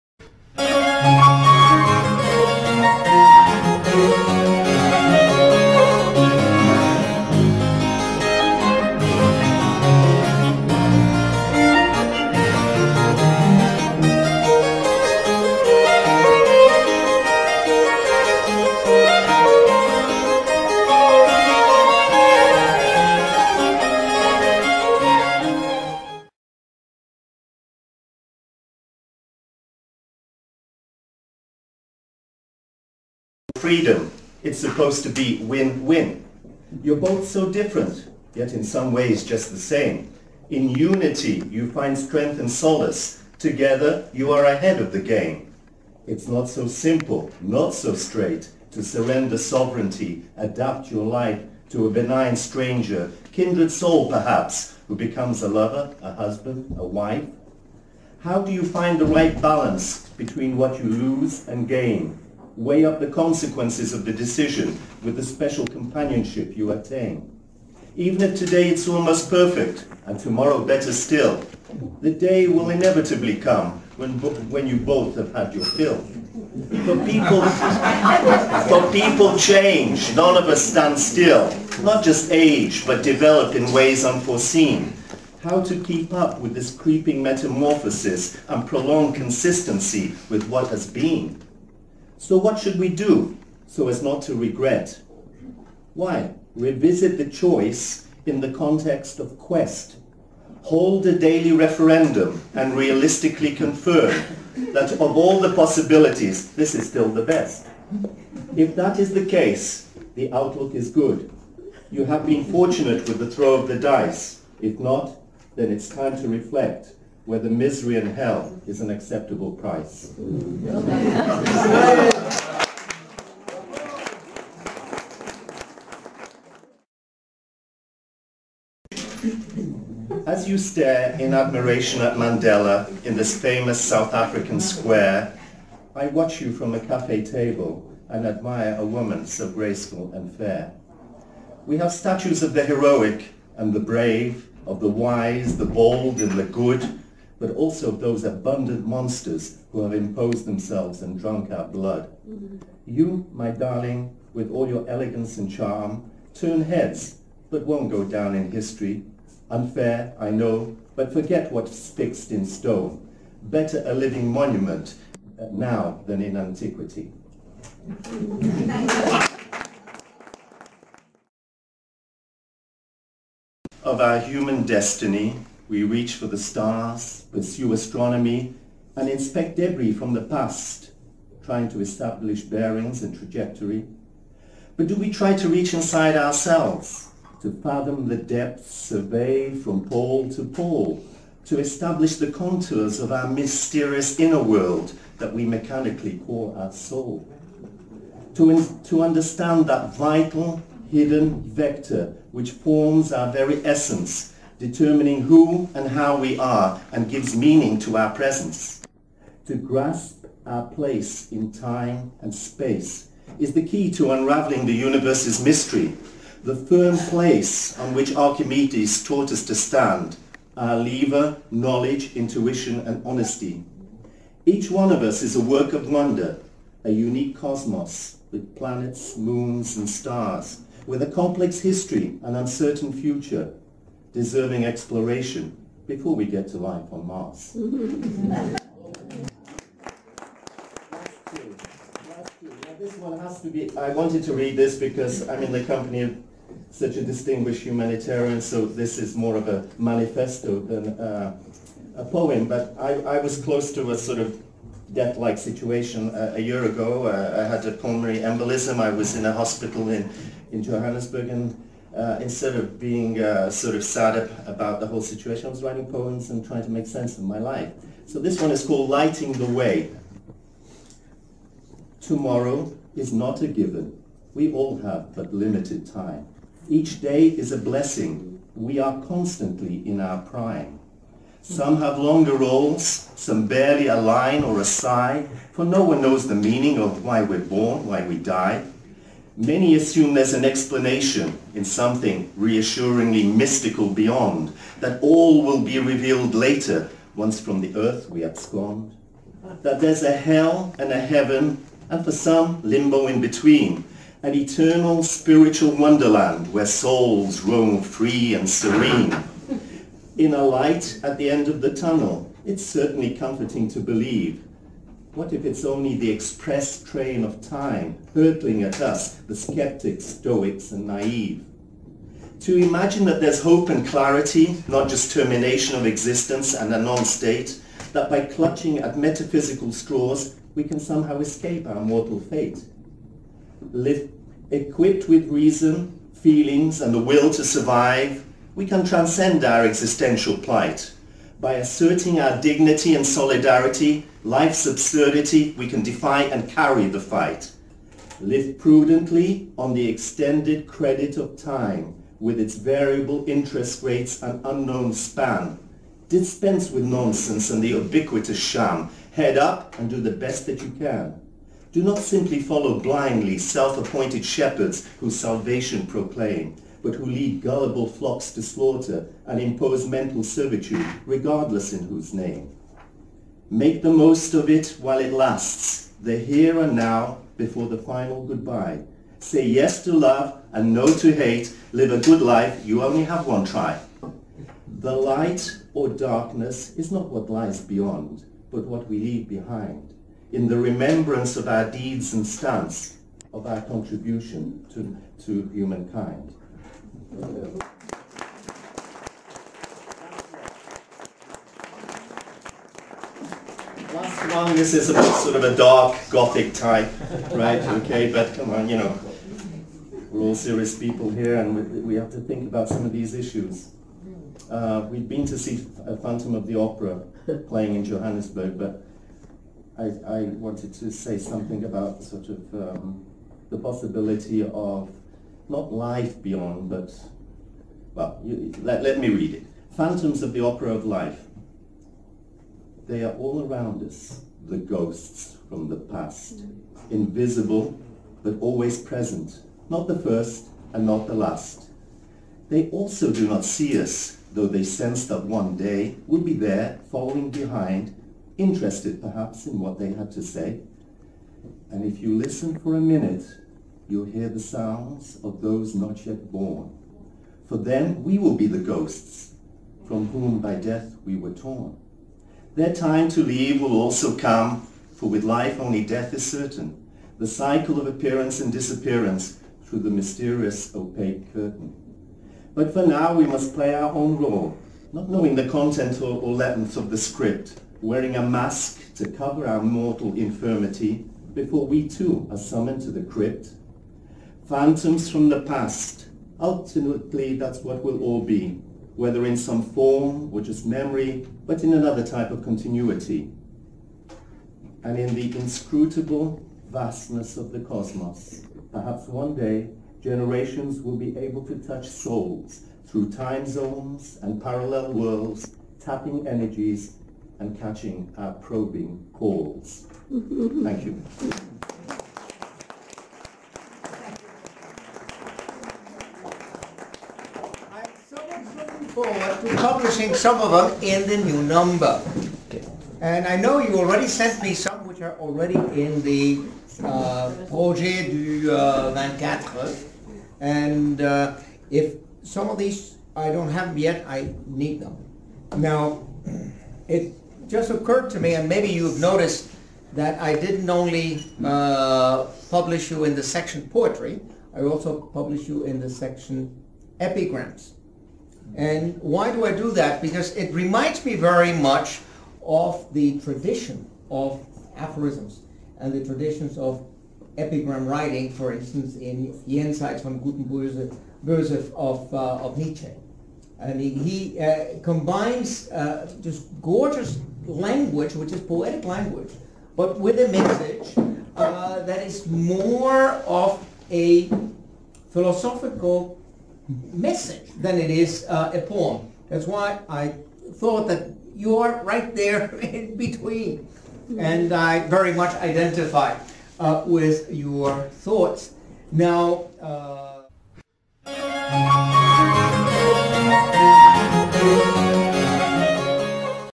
Bohdan Nahaylo reading his poems at Geneva UN Writers' Society evening, 25.01.2013
Bohdan Nahaylo reading some of his recent poems at a literary evening of the Geneva Writers' Society on 25 January 2013.  The event was hosted and chaired by Alfred de Zayas, the the co-founder and editor in chief of the Society's literary journal Ex Tempore.